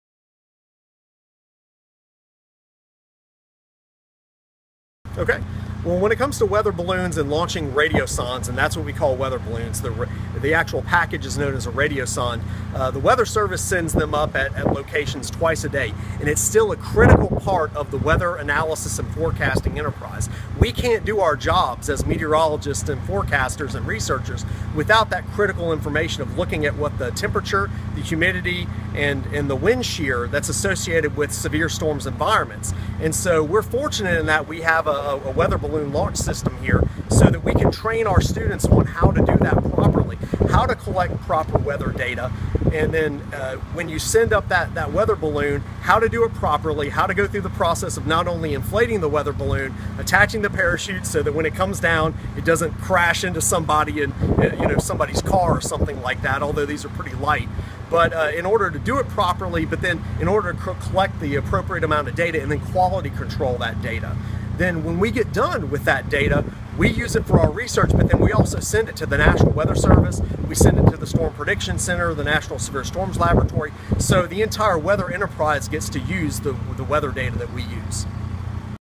Watch UAH Severe Weather Team Launch Weather Balloon
Students in the atmospheric science department at UAH launch a weather balloon 5.26.15 as a learning exercise before leaving for the Great Plains for a six-week research project studying nighttime thunderstorms.